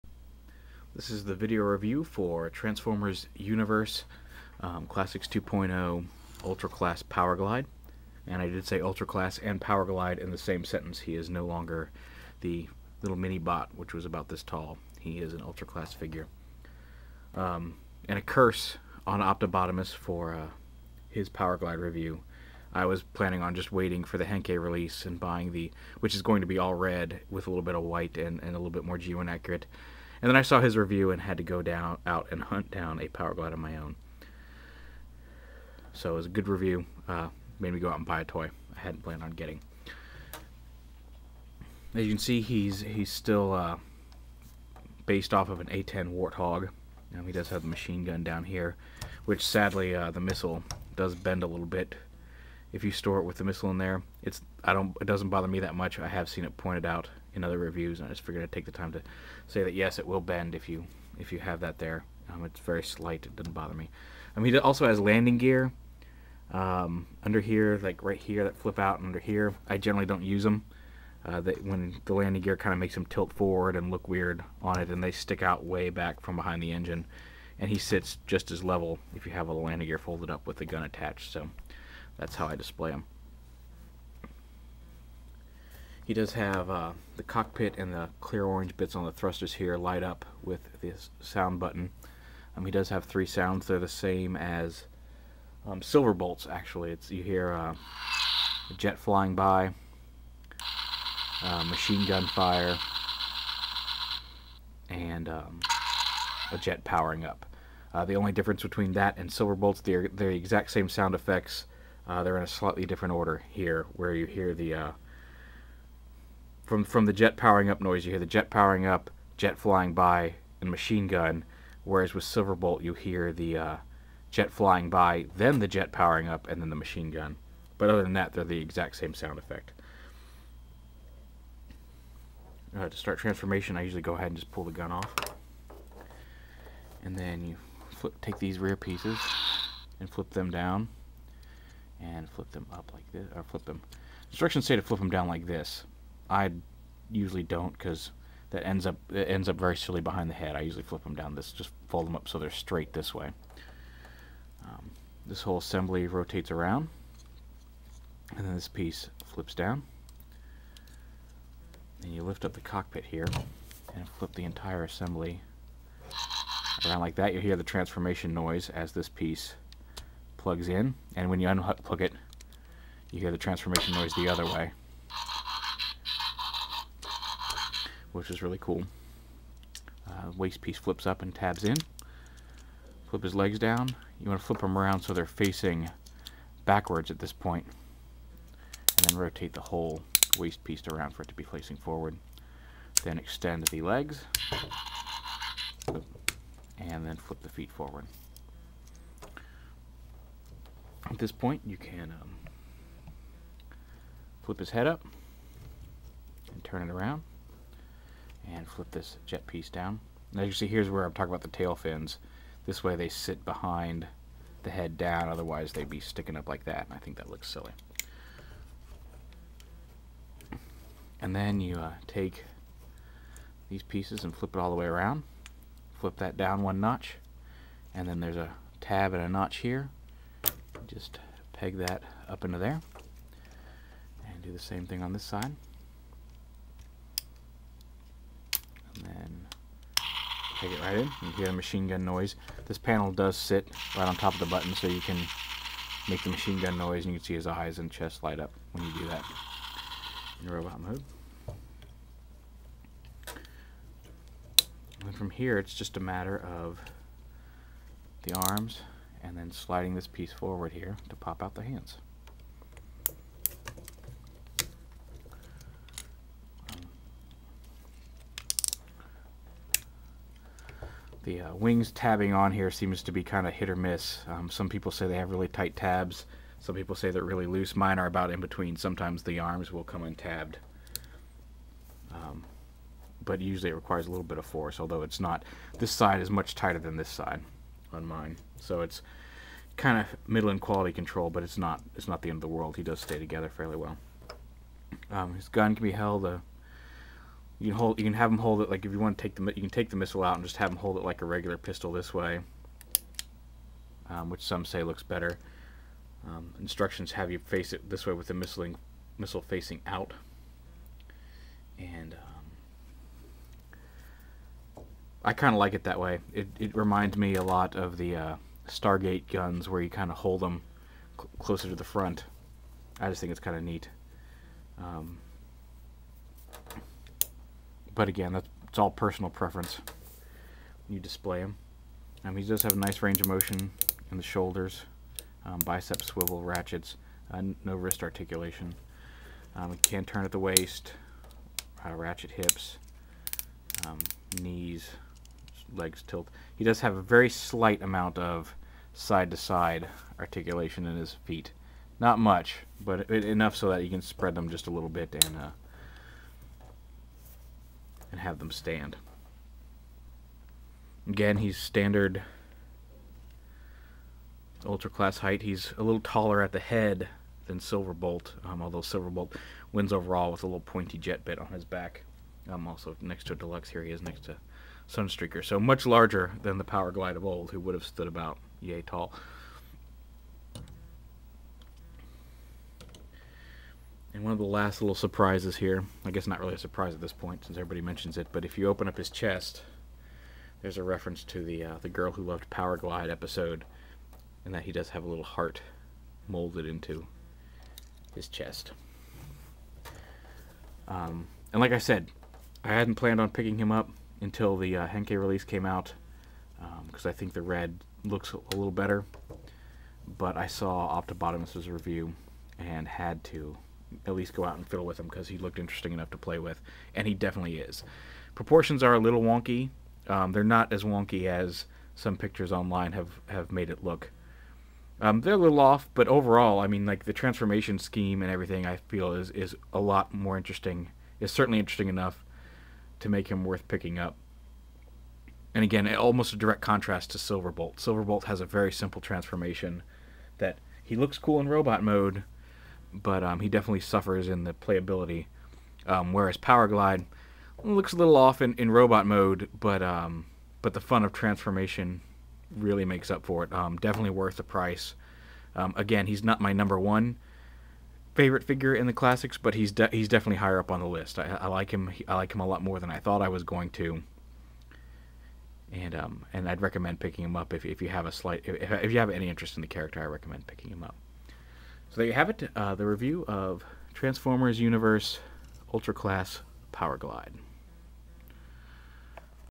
Transformers Universe/Classics 2.0 Powerglide
My review of Transformers Universe/Classics 2.0 Powerglide in which I show off and give my opinions on the figure.